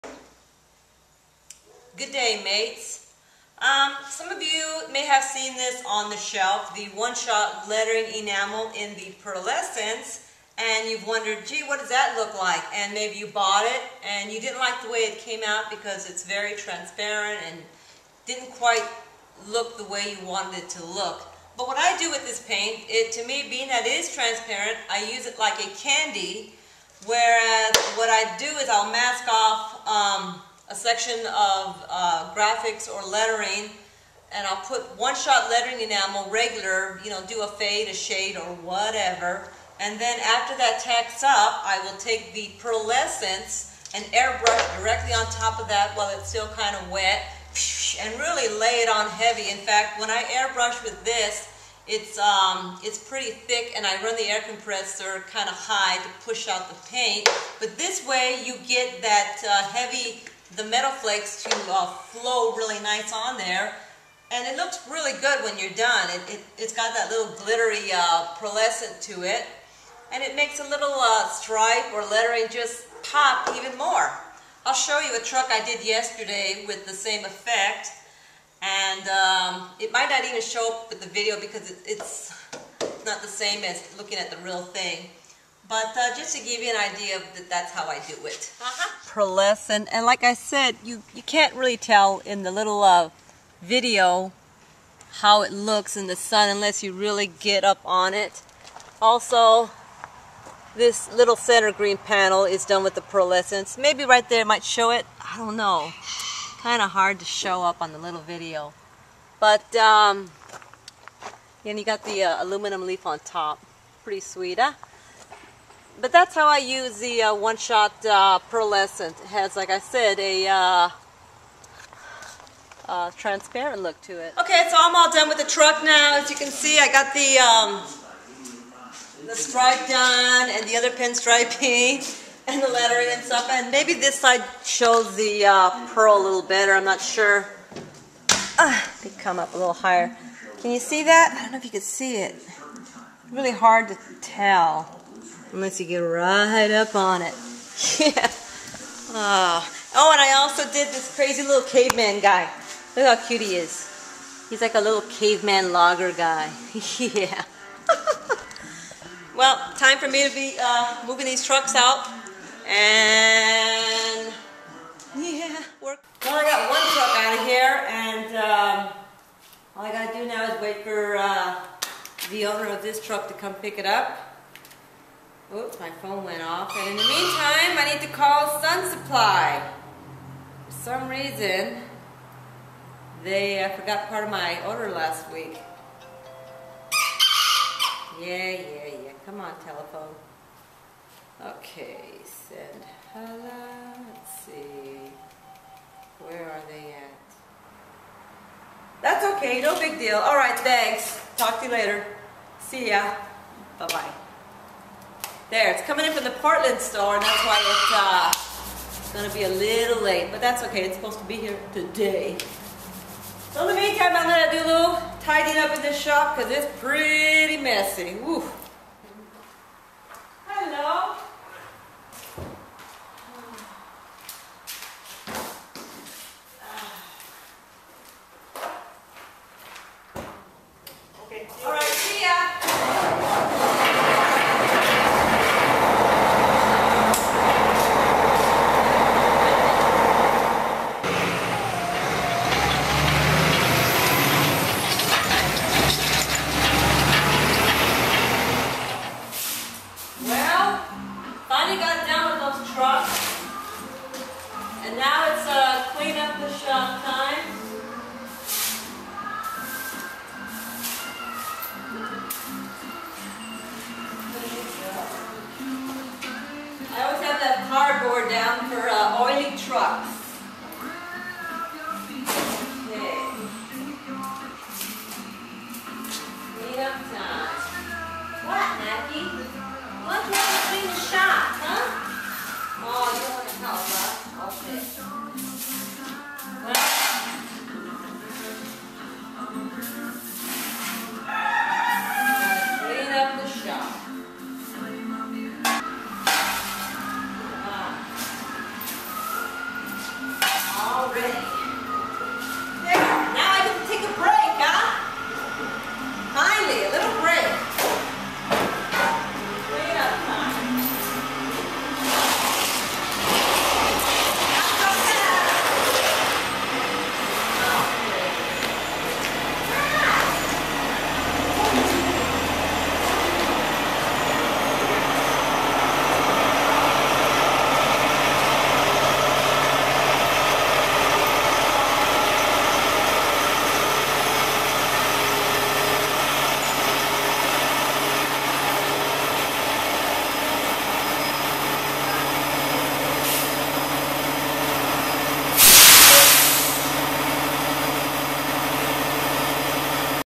Painting with 1-Shot Pearlescent
Here's a couple of trucks using 1-Shot Pearlescent Paint